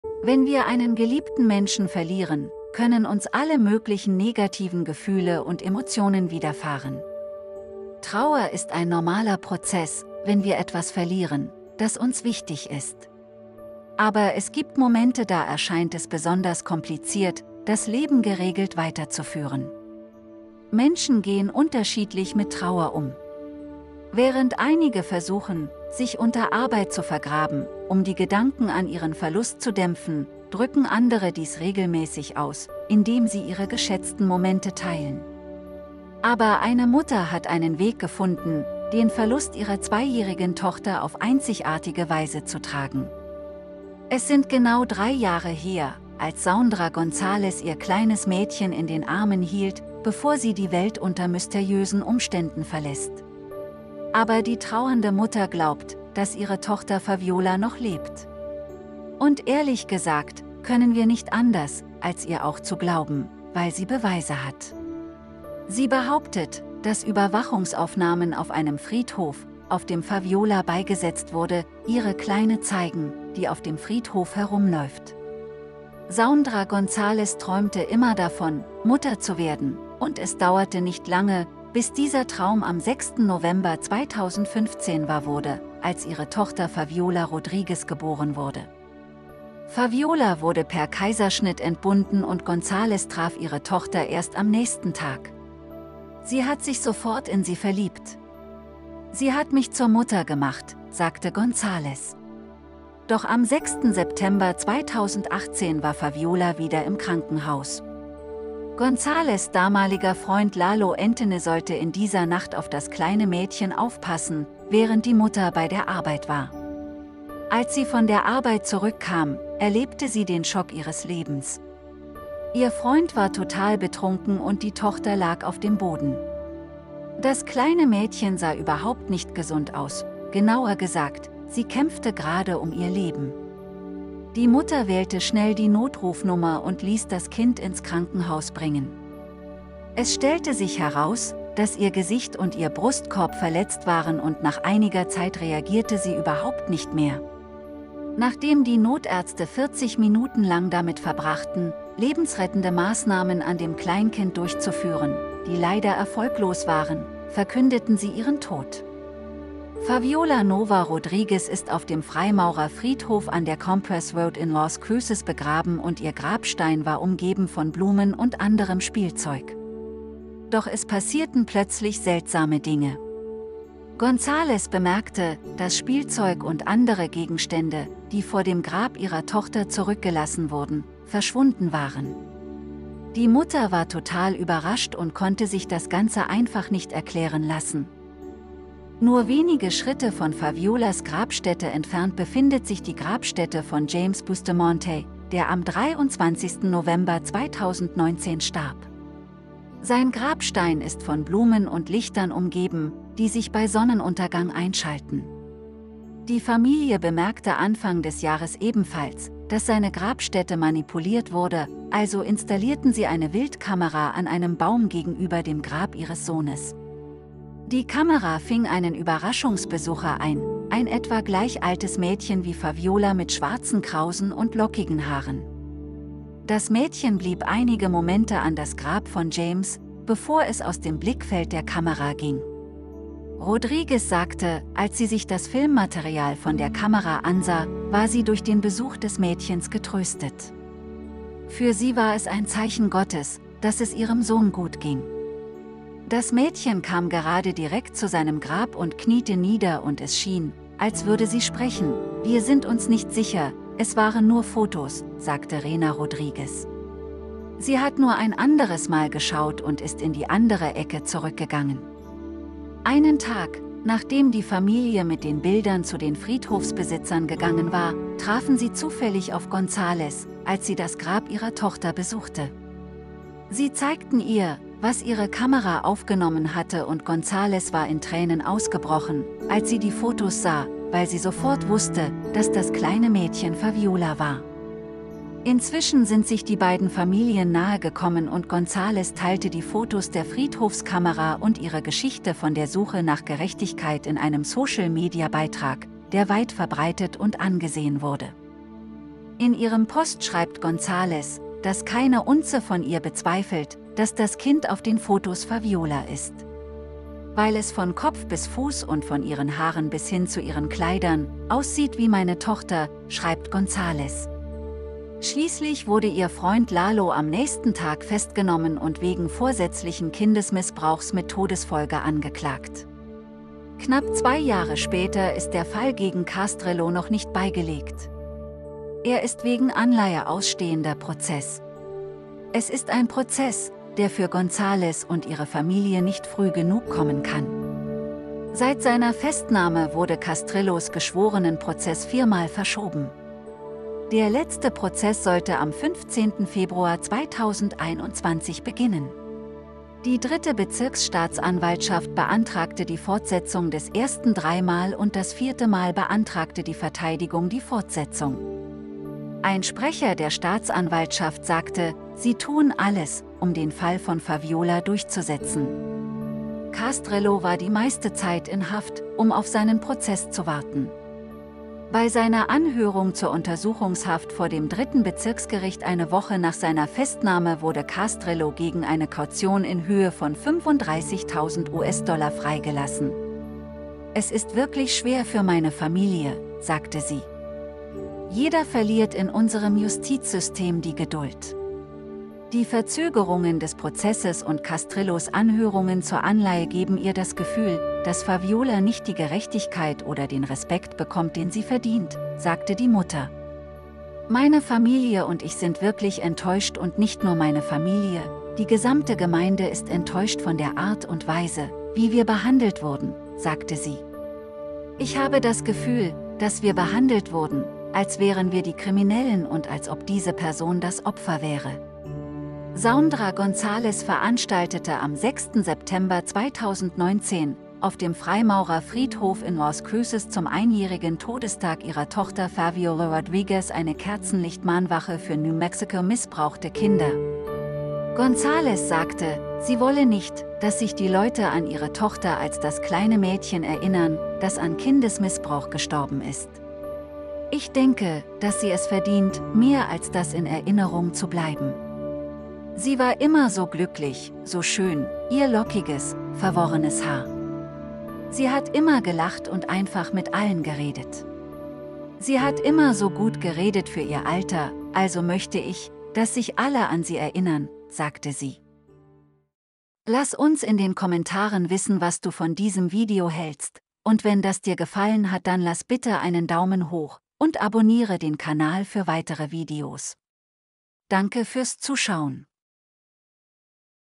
Kamera auf dem Friedhof zeigt, wie ein kleines Mädchen ihr eigenes Grab besucht!!! wahre Geschichte
Kamera auf dem Friedhof zeigt, wie ein kleines Mädchen ihr eigenes Grab besucht!!! Wenn wir einen geliebten Menschen verlieren, können uns alle möglichen negativen Gefühle und Emotionen widerfahren. Trauer ist ein normaler Prozess, wenn wir etwas verlieren, das uns wichtig ist. Aber es gibt Momente da erscheint es besonders kompliziert, das Leben geregelt weiterzuführen. Menschen gehen unterschiedlich mit Trauer um. Während einige versuchen, sich unter Arbeit zu vergraben, um die Gedanken an ihren Verlust zu dämpfen, drücken andere dies regelmäßig aus, indem sie ihre geschätzten Momente teilen. Aber eine Mutter hat einen Weg gefunden, den Verlust ihrer zweijährigen Tochter auf einzigartige Weise zu tragen.
Es sind genau drei Jahre her, als Saundra Gonzales ihr kleines Mädchen in den Armen hielt, bevor sie die Welt unter mysteriösen Umständen verlässt. Aber die trauernde Mutter glaubt, dass ihre Tochter Faviola Rodriguez noch lebt. Und ehrlich gesagt, können wir nicht anders, als ihr auch zu glauben, weil sie Beweise hat. Sie behauptet, dass Überwachungsaufnahmen auf einem Friedhof, auf dem Faviola beigesetzt wurde, ihre Kleine zeigen, die auf dem Friedhof herumläuft.

Hallo 👋  und herzlich willkommen auf unserem Kanal liebe Freunde.

Unsere Inhalte beziehen sich hauptsächlich auf wahre Geschichten aus der ganzen Welt.